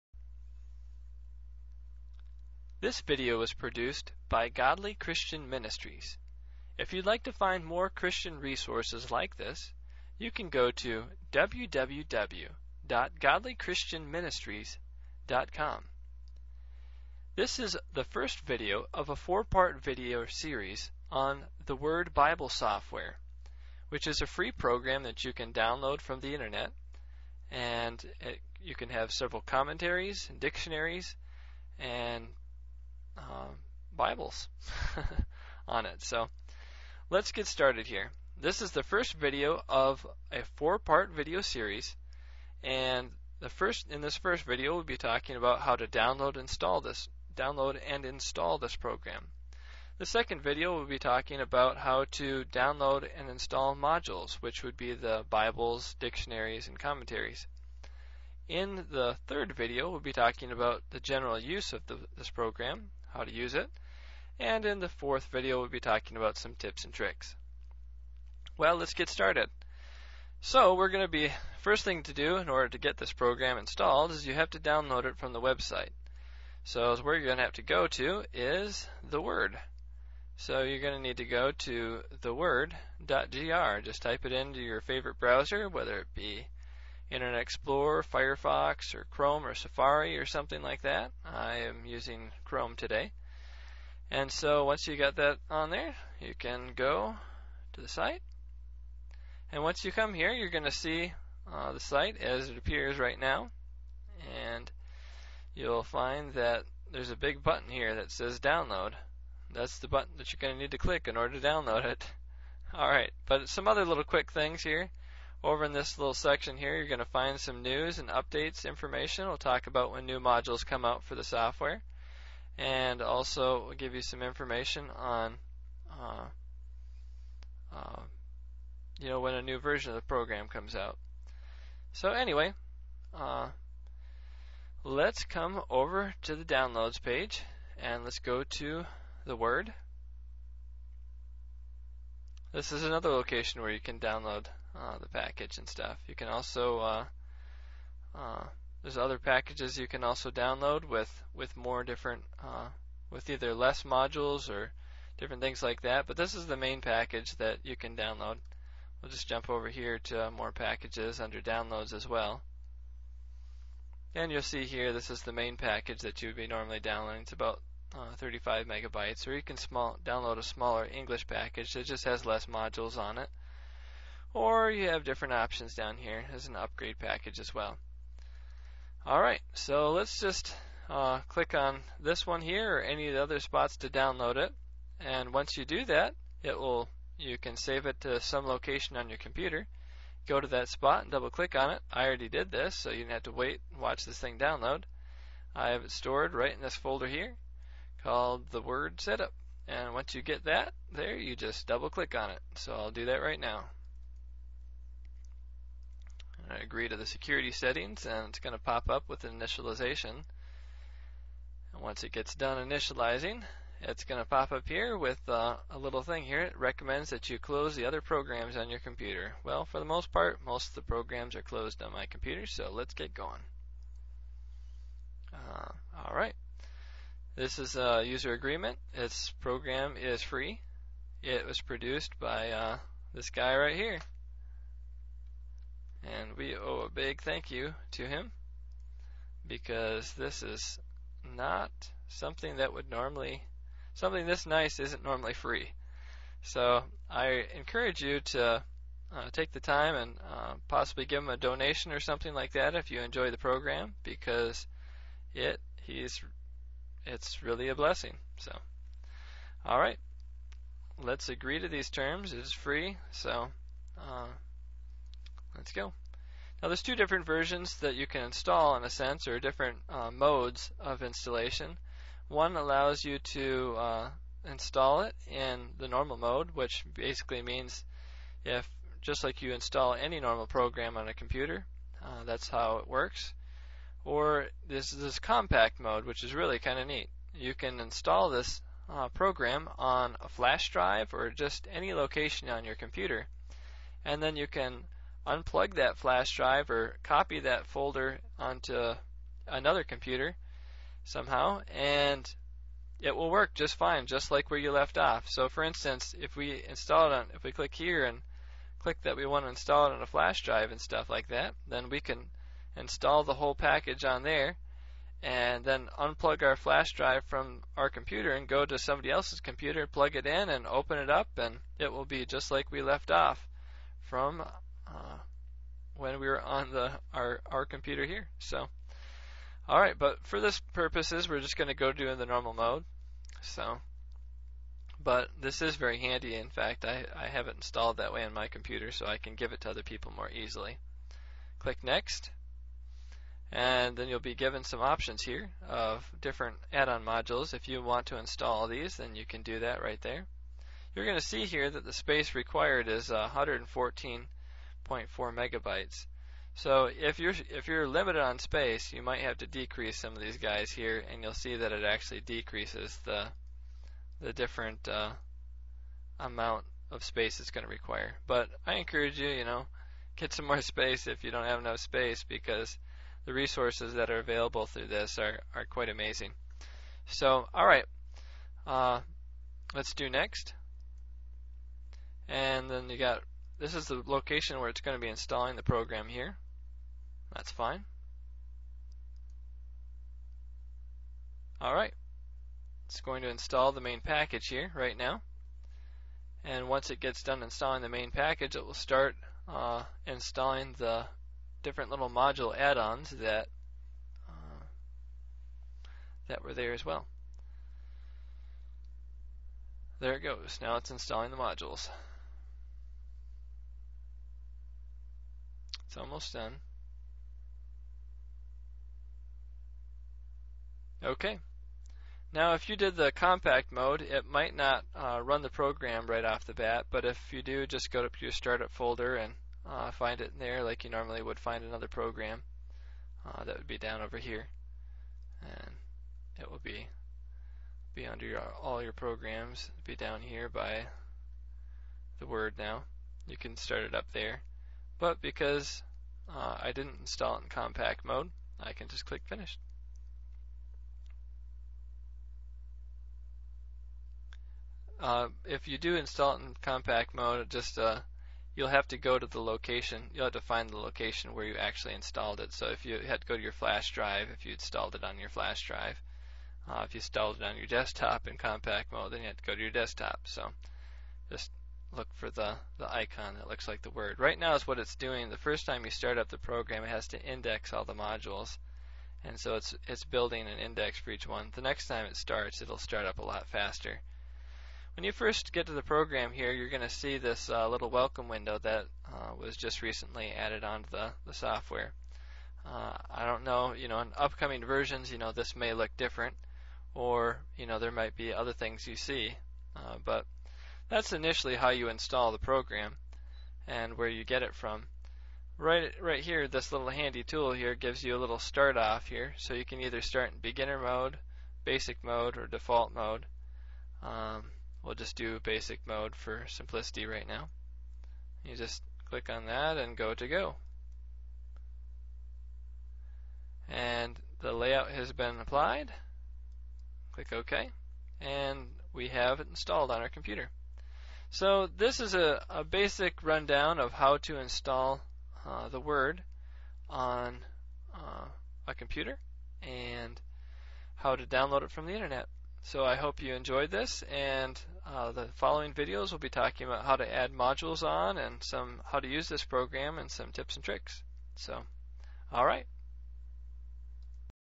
theWord Bible Software (1 of 4): Downloading and Installing
This is the 1st video of a 4 part series on theWord Bible Software.

This tutorial / instructional video covers how to download and install the main program.  You can download this program by going

This video was produced by Godly Christian Ministries... to find more resources like it you can go